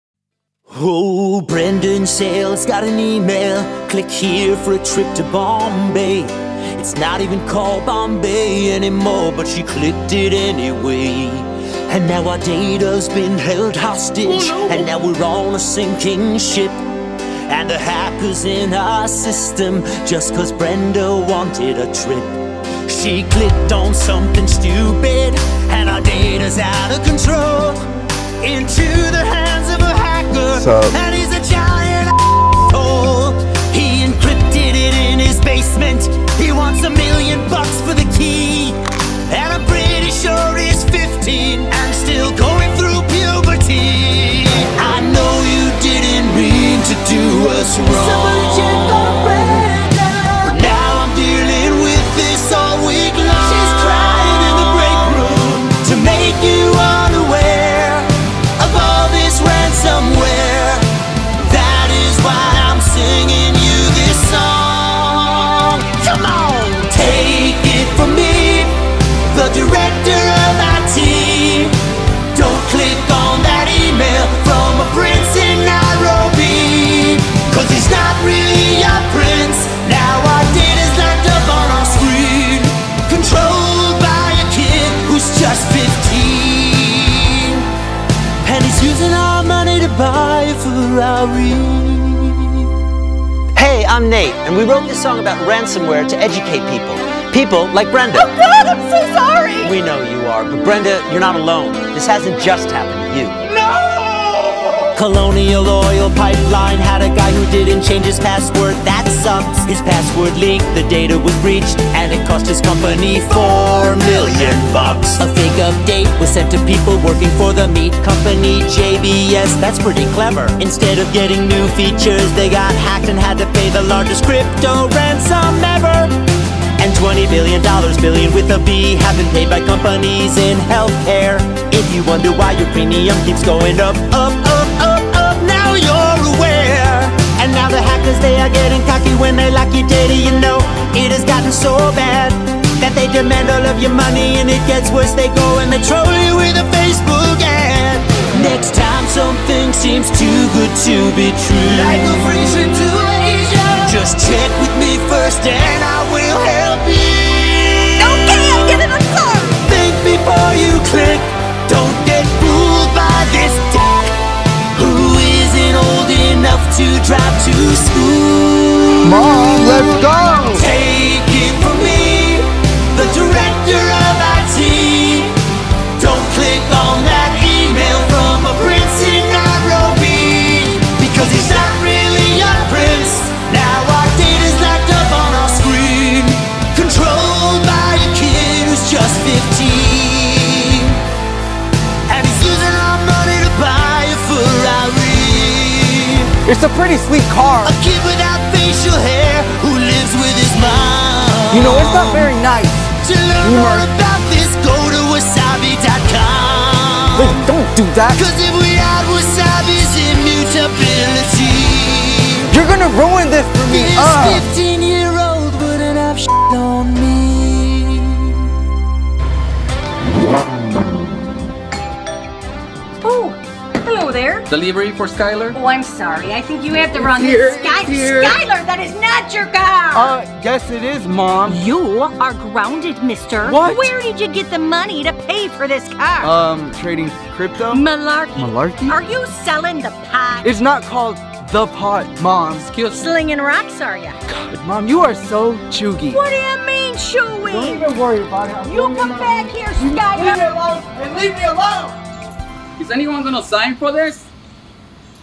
ThinkBeforeYouClick: Wasabi’s IT Hero Nate Returns to Warn About the Dangers of Ransomware
Ransomware can be lurking anywhere.  Education is the best defense against attacks. But when that fails, there’s always Wasabi.

Don’t let the cybercriminals win.  With cybercriminals extorting millions of dollars from organizations every year, ransomware is not going away any time soon. Protect yourself with proven security best practices, including regular ransomware awareness training and a robust backup and recovery strategy with immutable cloud storage.

Click happens. But you don’t have to lose your data over it.

• In a world where ransomware has become a regular problem, Immutable Storage is emerging as a solution to mitigate threats.
• Wasabi's Immutable Buckets and support for S3 object-lock ensures data security and integrity.
• The industry standard for backup and recovery calls for one offsite copy that is immutable, providing 100% data integrity over a predefined retention period.
• Object-level immutability gives the highest level of security to your data storage.